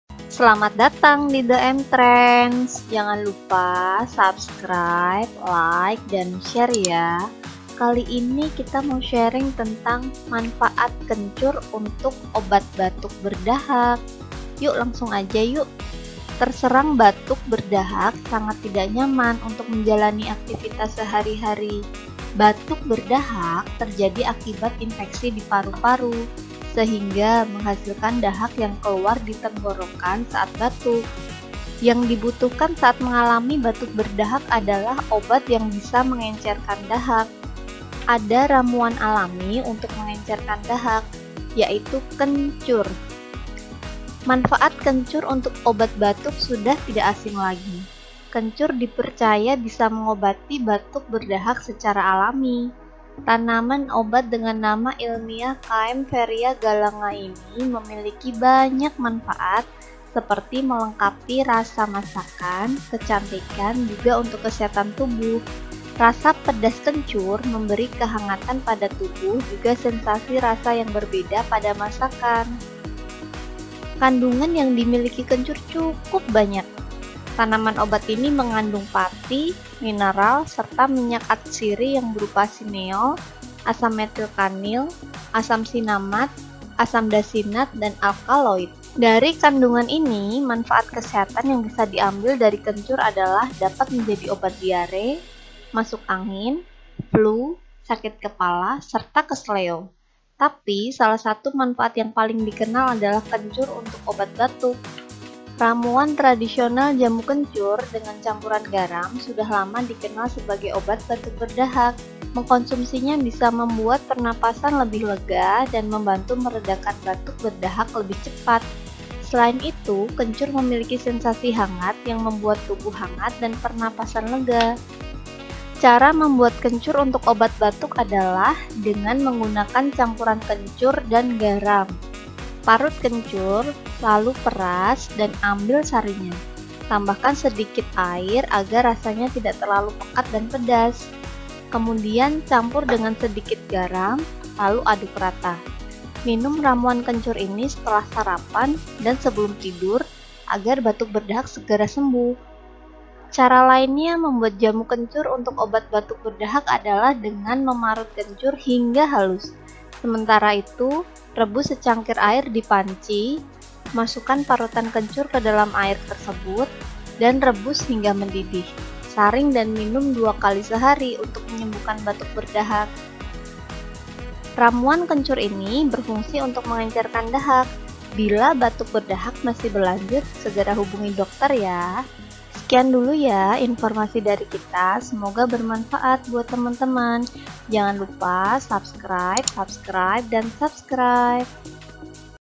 LUAR BIASA !!! Manfaat Kencur untuk Batuk Berdahak
Terserang batuk berdahak sangat tidak nyaman untuk menjalani aktifitas sehari-hari. Batuk berdahak terjadi akibat infeksi di paru-paru, sehingga menghasilkan dahak yang keluar di tenggorokan saat batuk. Yang dibutuhkan saat mengalami batuk berdahak adalah obat yang bisa mengencerkan dahak. Ada ramuan alami untuk mengencerkan dahak, yaitu kencur.

Khasiat kencur untuk obat batuk sudah tidak asing lagi.  Kencur dipercaya bisa mengobati batuk berdahak secara alami. Tanaman obat dengan nama ilmiah Kaempferia galanga ini memiliki banyak manfaat seperti melengkapi rasa masakan, kecantikan, juga untuk kesehatan tubuh. Rasa pedas kencur memberi kehangatan pada tubuh juga sensasi rasa yang berbeda pada masakan.

Kandungan yang dimiliki kencur cukup banyak. Tanaman obat ini mengandung pati, mineral, serta minyak atsiri yang berupa sineol, asam metil kanil, asam sinamat, asam dasinat dan alkaloid. Dari kandungan ini, manfaat kesehatan yang bisa diambil dari kencur adalah dapat menjadi obat diare, masuk angin, flu, sakit kepala, serta keseleo. Tapi salah satu manfaat yang paling dikenal adalah kencur untuk obat batuk.

Ramuan tradisional jamu kencur dengan campuran garam sudah lama dikenal sebagai obat batuk berdahak. Mengonsumsinya bisa membuat pernapasan lebih lega dan membantu meredakan batuk berdahak lebih cepat. Selain itu, kencur memiliki sensasi hangat yang membuat tubuh hangat dan pernapasan lega.

Cara membuat kencur untuk obat batuk adalah dengan menggunakan campuran kencur dan garam. Parut kencur, lalu peras dan ambir sarinya. Tambahkan sedikit air agar rasanya tidak terlalu pekat dan pedas. Kemudian, campur dengan sedikit garam, lalu aduk rata. Minum ramuan kencur ini setelah sarapan dan sebelum tidur agar batuk berdahak segera sembuh.

Cara lainnya membuat jamu kencur untuk obat batuk berdahak adalah dengan memarut kencur hingga halus. Sementara itu, rebus secangkir air di panci. Masukkan parutan kencur ke dalam air tersebut, dan rebus hingga mendidih. Saring dan minum dua kali sehari untuk menyembuhkan batuk berdahak.

Ramuan kencur ini berfungsi untuk mengencerkan dahak. Bila batuk berdahak masih berlanjut, segera hubungi dokter.